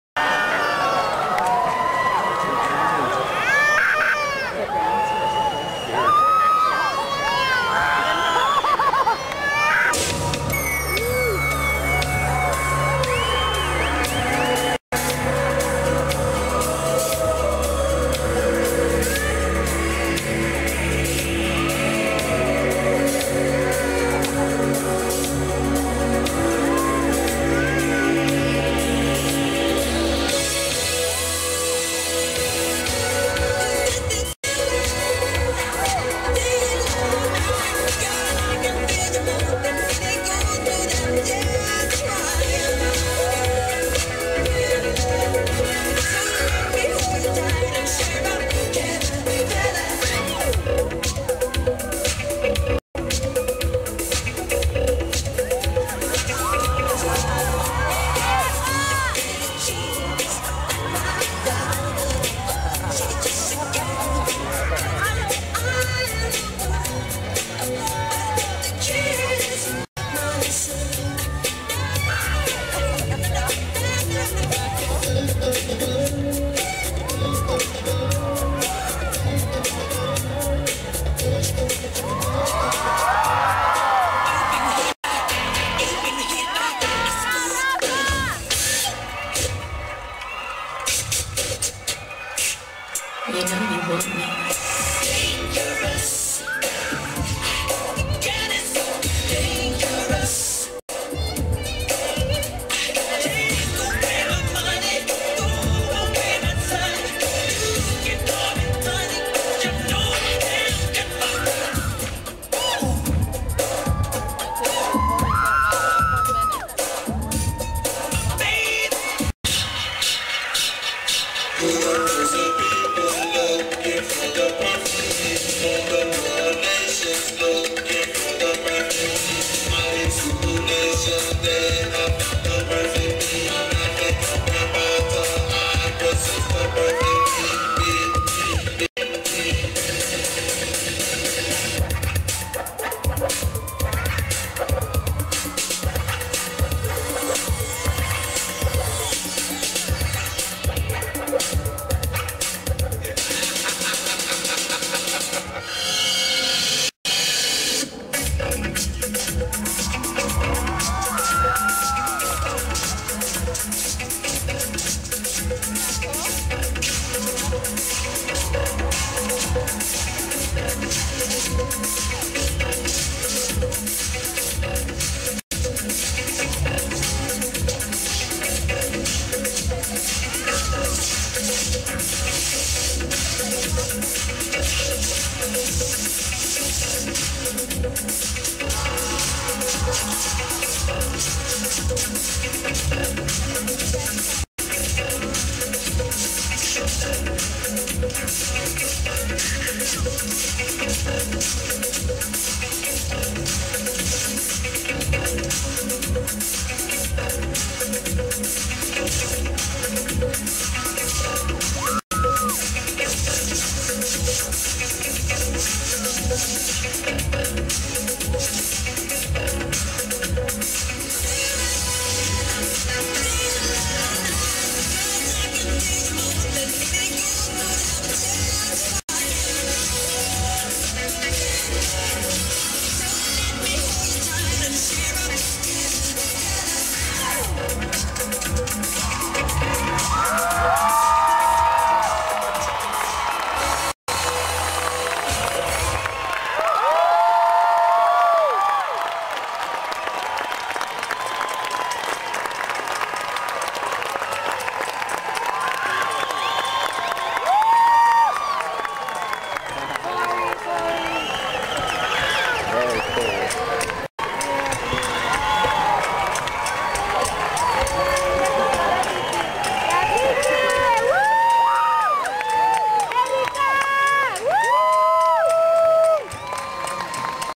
Groupo De West LA Dancing A Michael Jackson Tribute Dance
John Estrada's groupo de west la is dance their michael jackson tribute dance at the courthouse in santa barbara's old spanish fiesta days 2009. viva la fiesta